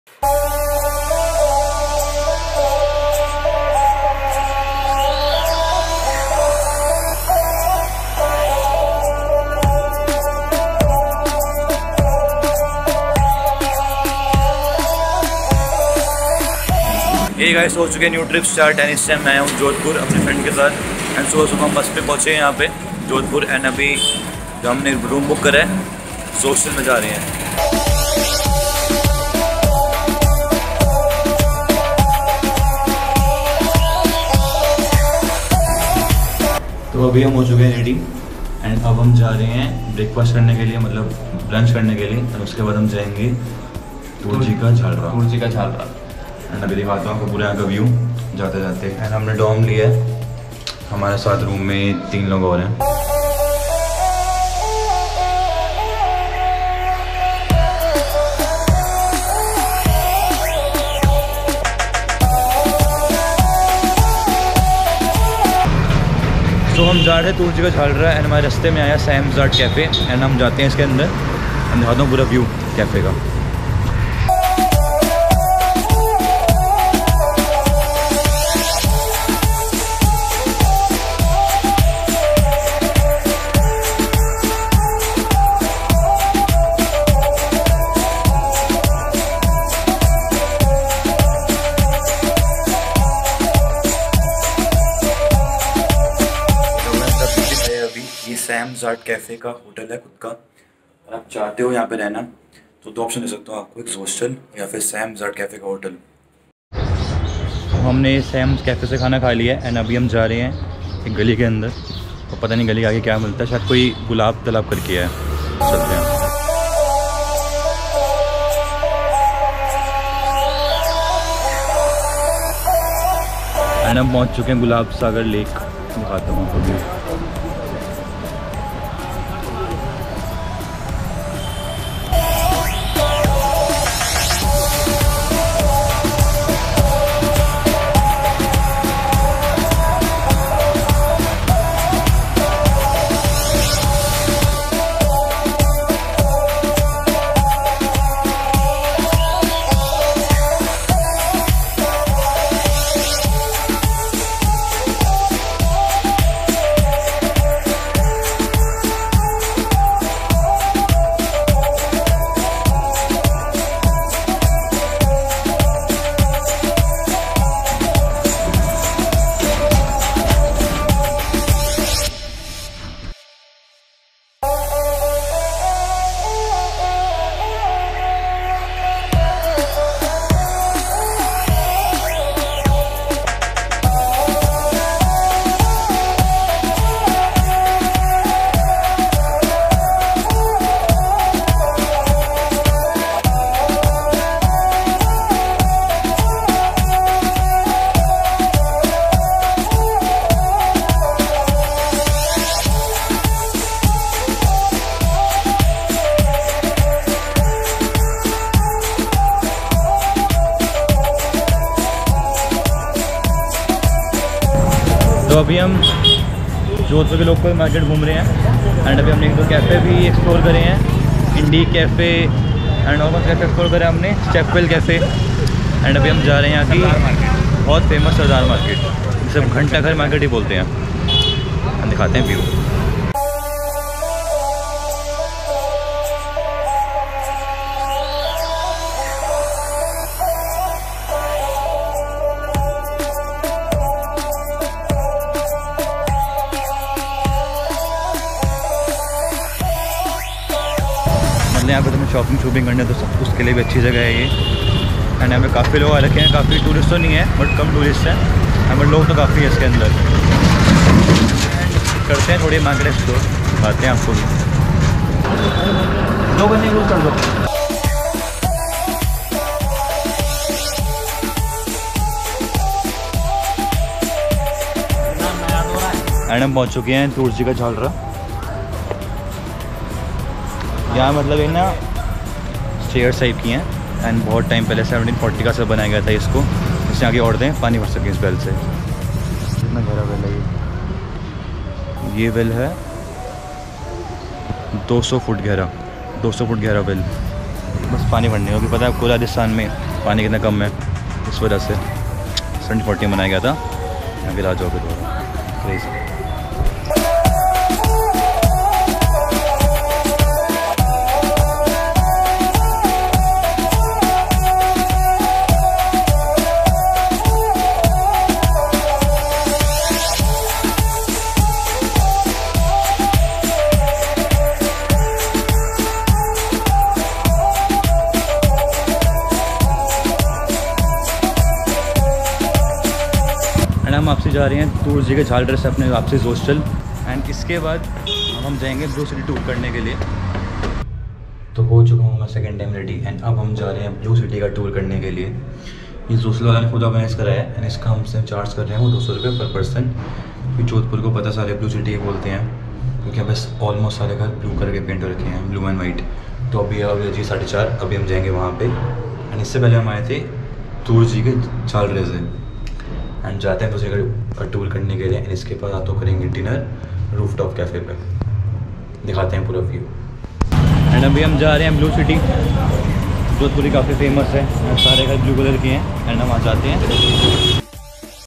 ZOSTEL JODHPUR | Travel Vlog | Places and Cafes To Visit & See | The Complete Travel Guide
I booked a bus from Delhi till Jodhpur in 1000 rupees, it was a 12 hours journey, then we stayed in Zostel which cost us 400/- per person in dorms.
Then we visited some cafes, I personally liked the 4 cafes which are, Zostel's rooftop cafe, Indique Cafe, Step Well Cafe, and Sam's art cafe.

In this video, I want to give you a virtual tour of Jodhpur.  This video will also serve as a Jodhpur Travel Guide. This video has all the information you need to travel to Jodhpur.

This video will answer questions like

1. How to reach Jodhpur?
2. Places to stay in Jodhpur?
3. Places to see in Jodhpur?

4. Places to eat in Jodhpur?